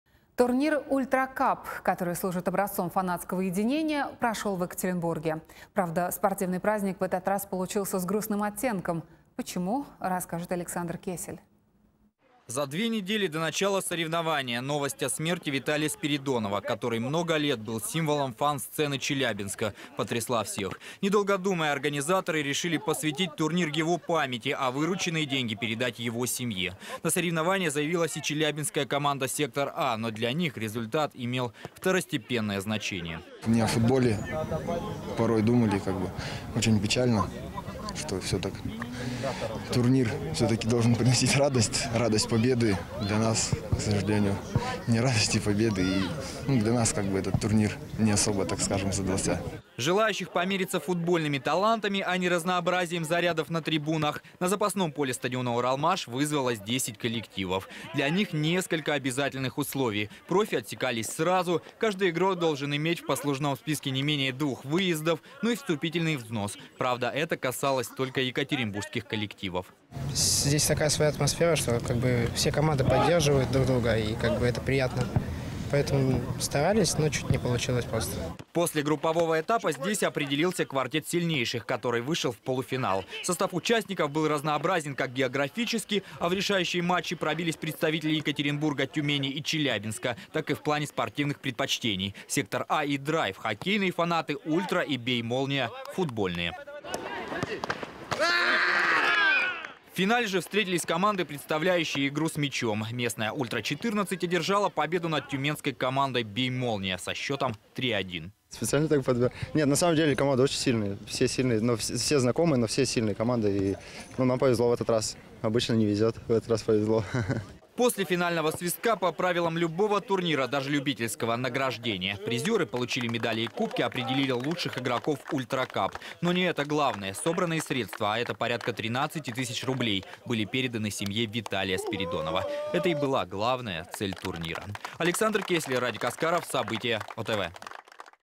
Турнир «Ультракап» в Екатеринбурге посвятили памяти челябинского фаната
Турнир «Ультракап», который служит образцом фанатского единения, прошел в Екатеринбурге. Правда, спортивный праздник в этот раз получился с грустным оттенком.